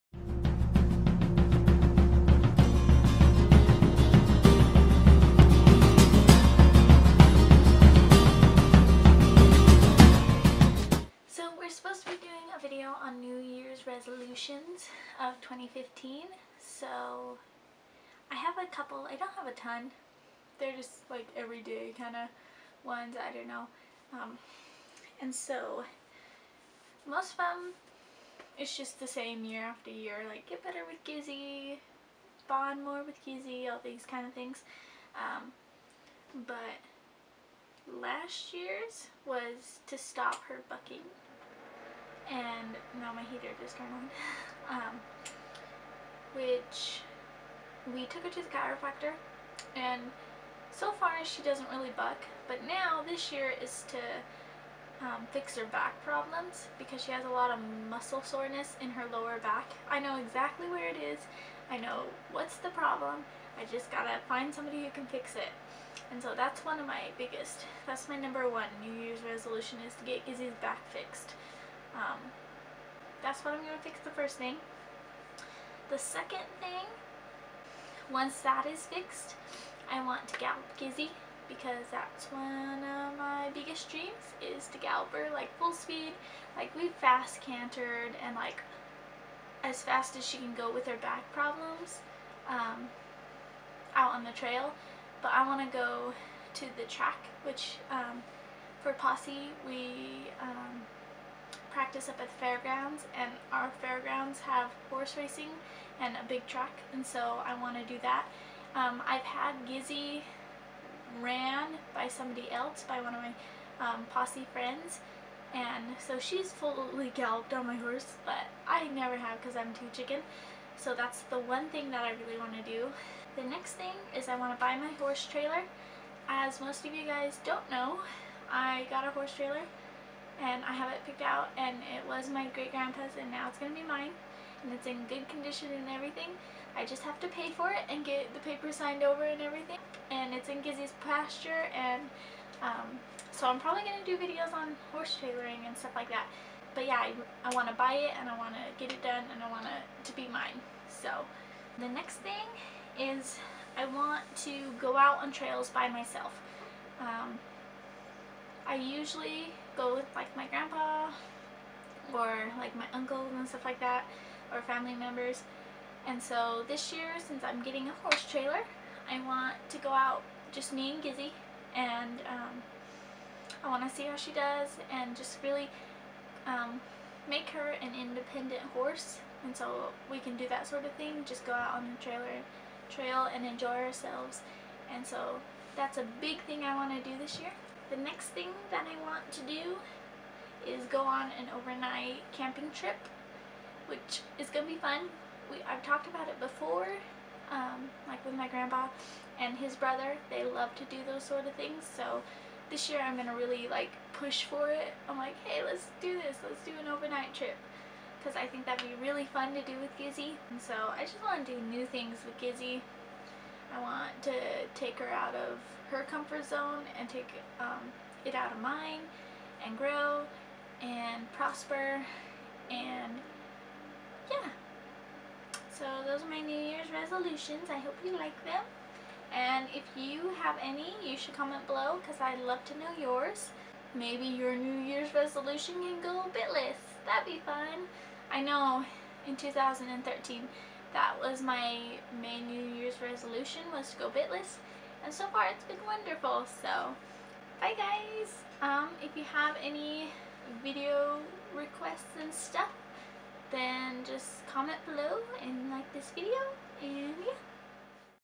2k15 New Year's Resolutions
So here's my resolutions of this year and im excited about it and ready to start this journey- kelsey